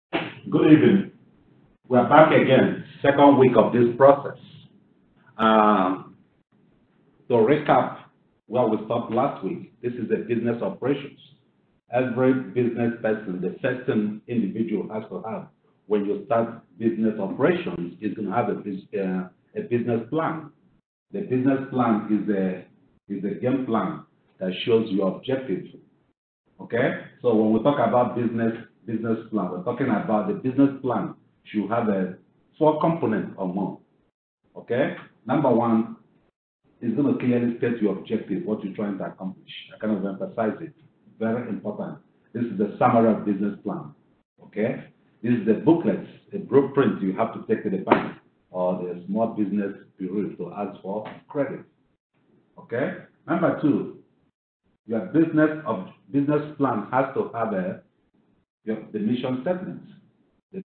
Martin Offiah Class Instruction
This is a short video of Professor Martin Offiah during Introduction to Business and Technology Class instruction at DeVry University, Sherman Oaks, California location.